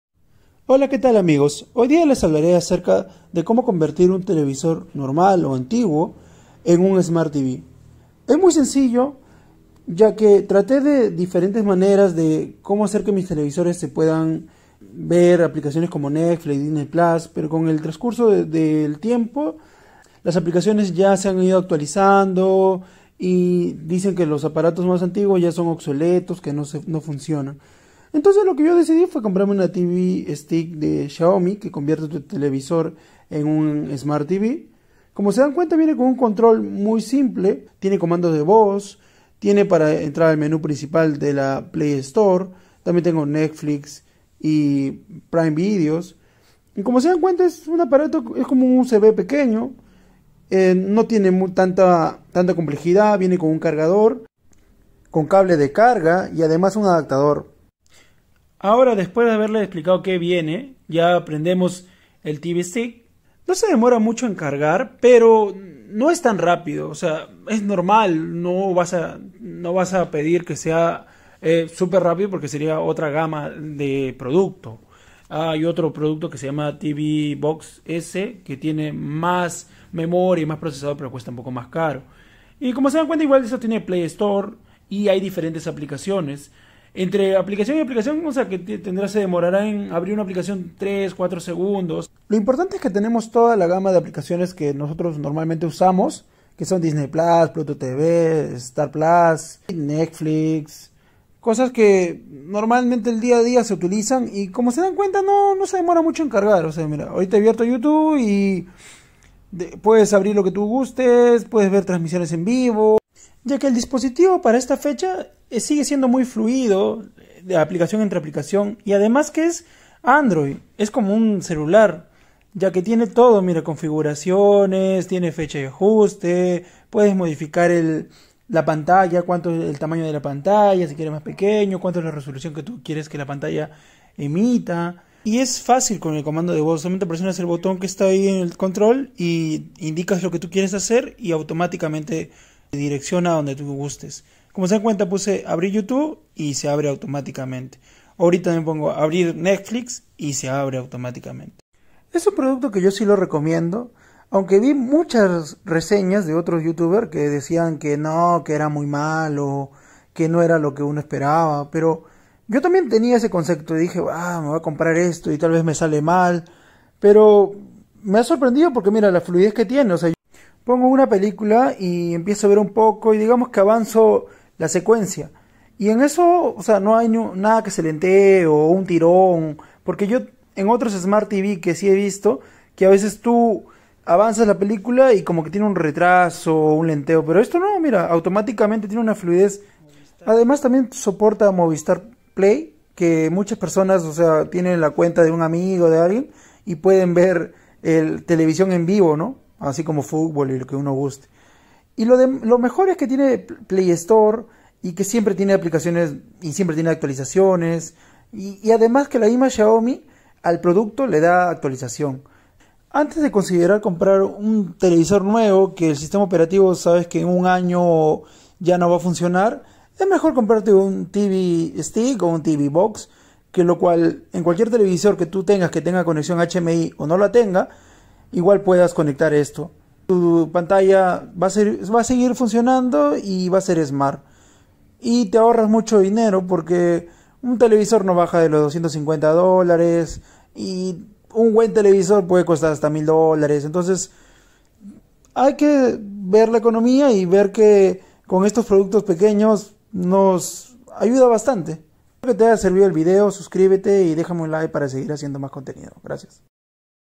Convertir cualquier tv en Smart tv
hola qué tal amigos hoy le explicó y muestro que tal es tener un televisor antiguo o normal sin smart.tv y convertirlo a Smart tv. el precio de tv stick de Xiaomi está en diferentes mercados desde los 32 dólares hasta los 45 dólares . en caso de Perú en la misma tienda de Xiaomi está 179 soles aunque en otras tiendas como coolbox Vi que costaba 256 soles . y mercado libre que estaba 150 soles y en otros mercados en 32 dólares otros. 35 dólares.